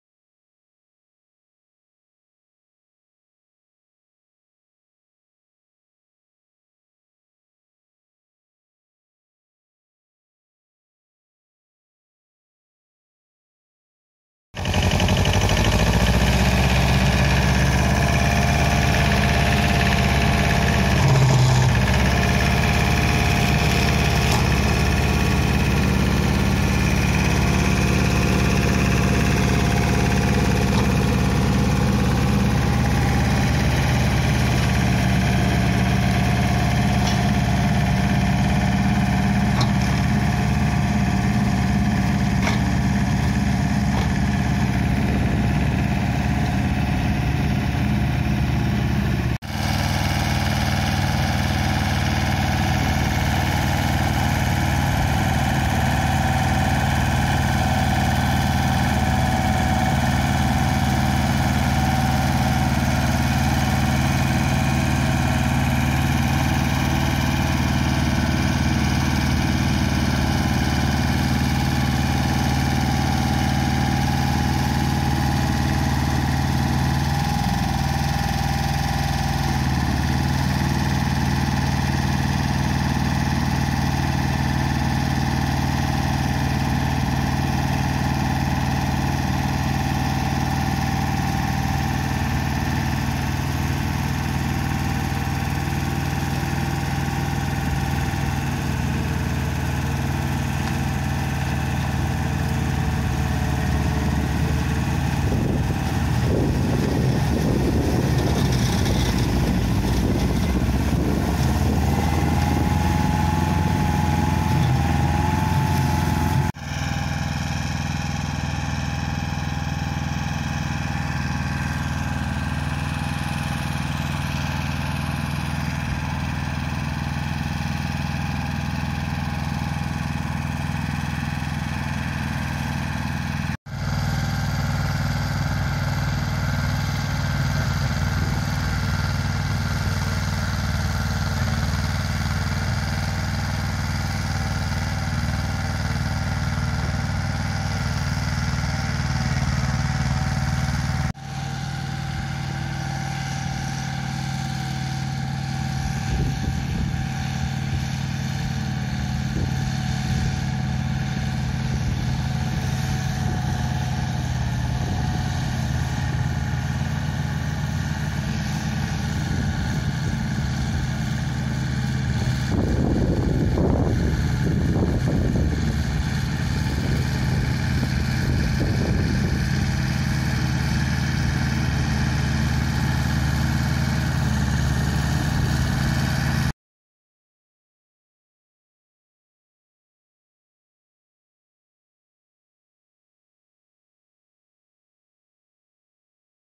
Taglio del sovescio e di rovi
Trattore Pasquali 997 (30CV) e trincia Agrimaster Yoyo